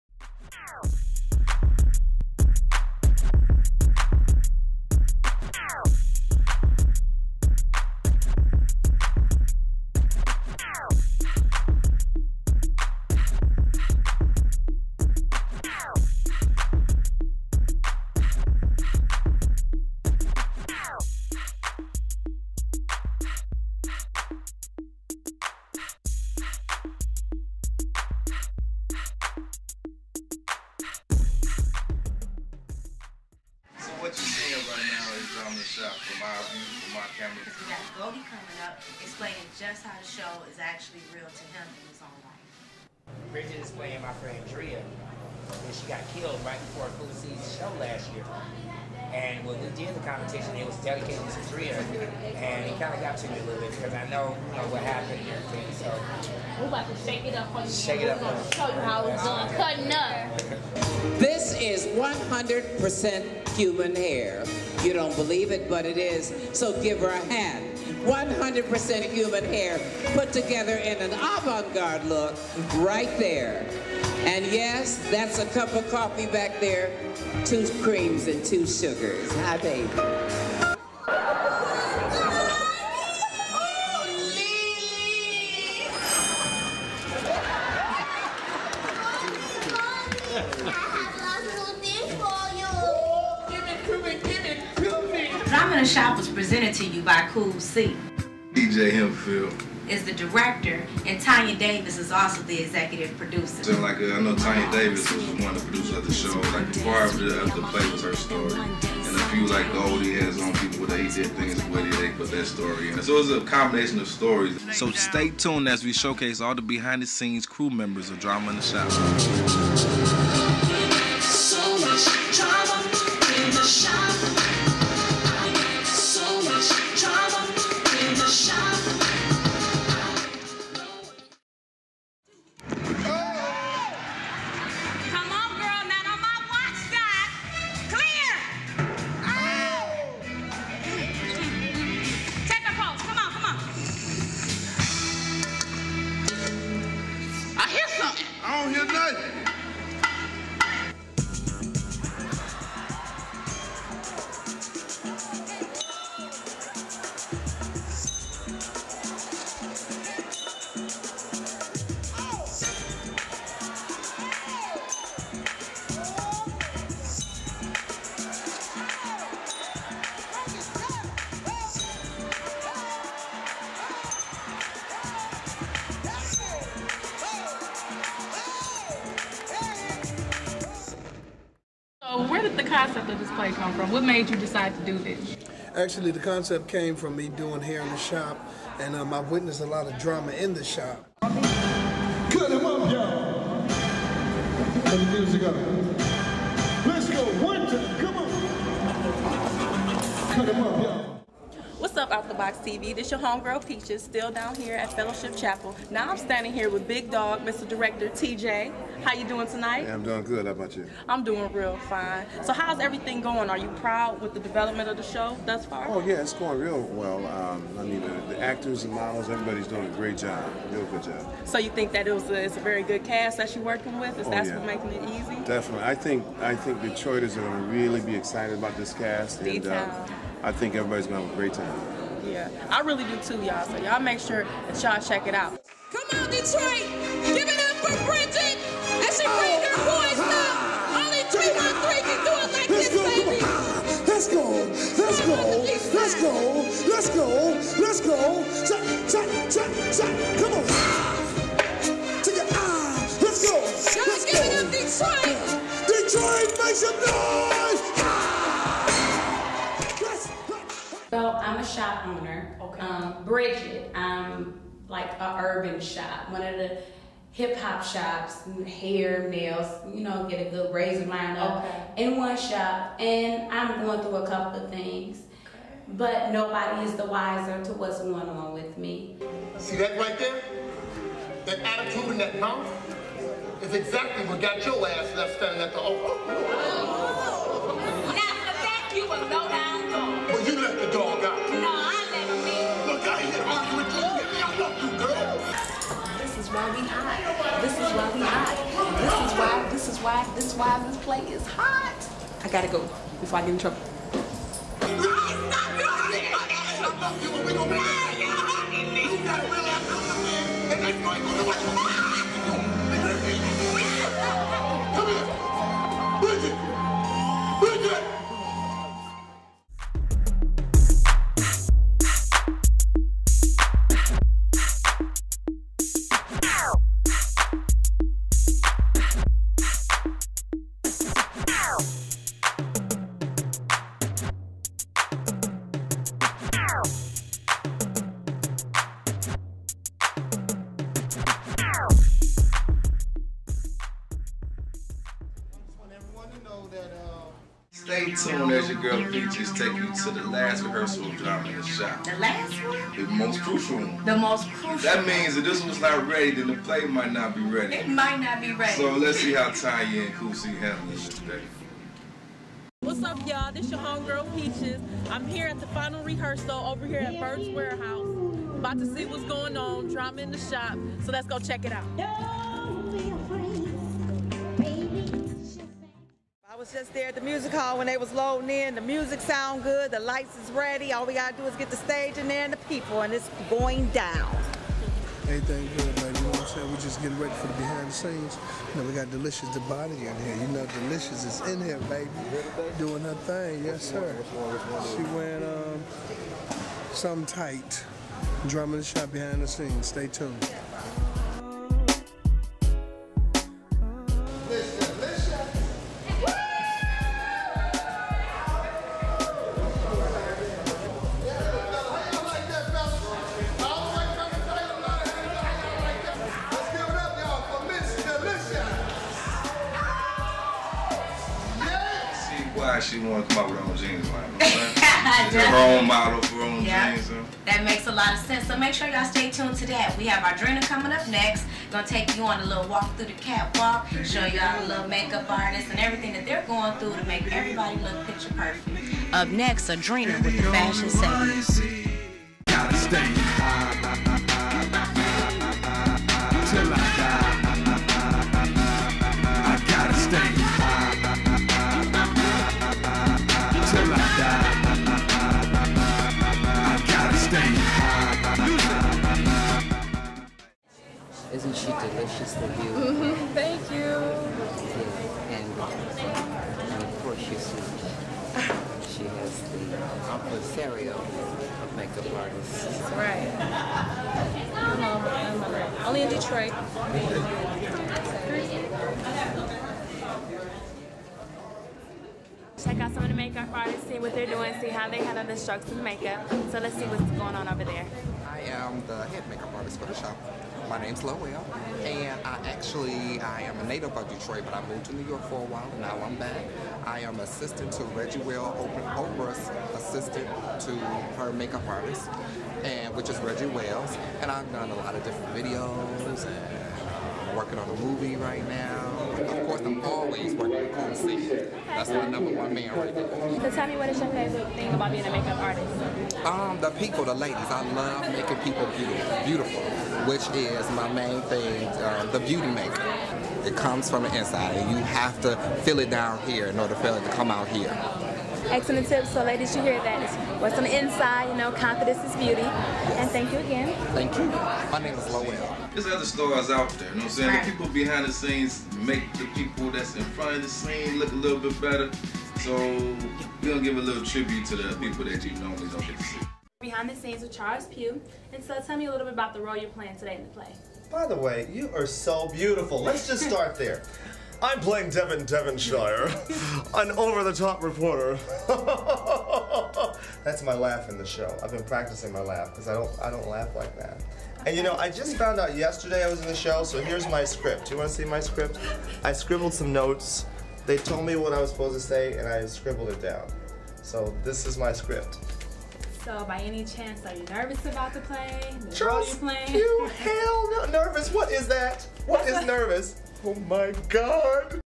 Out Tha Box T.V. (Behind the Drama) Pt. 2 Episode # 8
Out Tha Box T.V. steps into the "DRAMA" with Cool "C" World Renowned Hair Conesuer from Detroit Mich. and his Talented, Star studded Celebrity cast of the Stage Play "Drama in the Shop" Directed by T.J. Hemphill and Executive Produced by Tanya Davis, Watch all 3 Clips to see Exclusive interview with A.J. Johnson, Hot Performances by Detroit Artist Stretch Money, Detroit's Hottest Hair Shows/Battles and to witness the Excitement of presenting a major Stage Play!